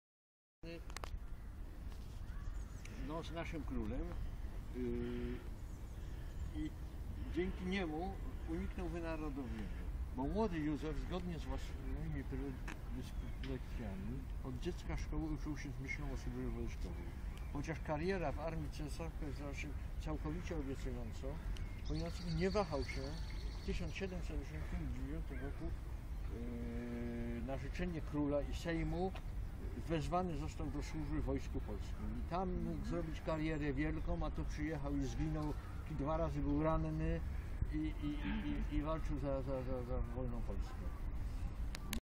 S@S SZKOLA AKTYWNEGO SENIORA
EDUKACYJNA LEKCJA W PARKU JORDANA W KRAKOWIE